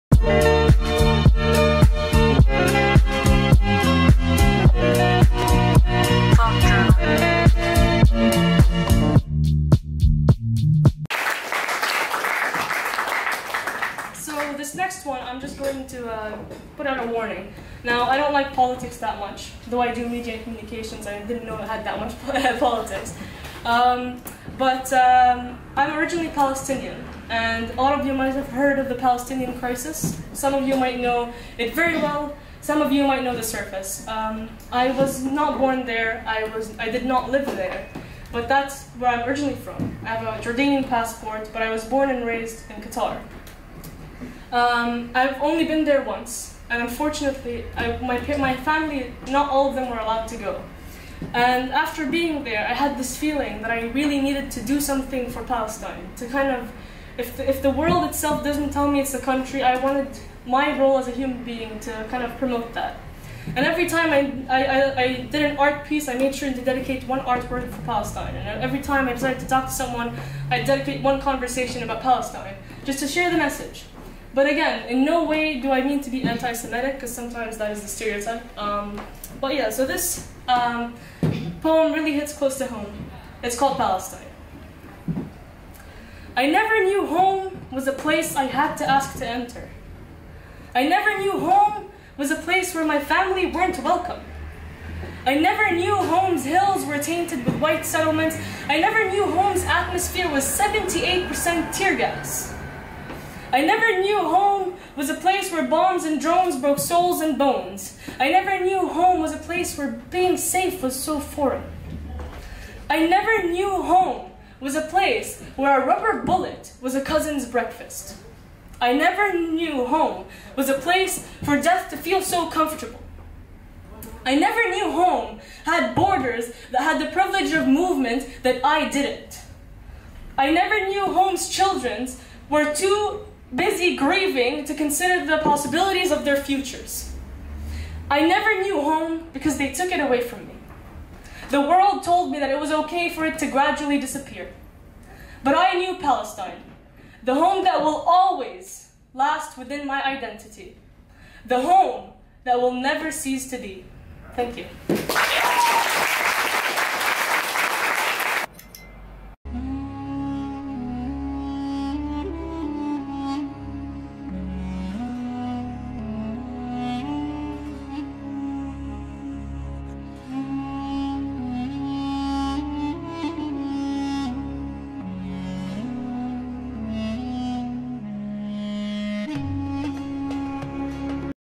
A POEM FOR PALESTINE
This is a poem that I wrote for and about Palestine, where I'm originally from. I hope you guys enjoy it.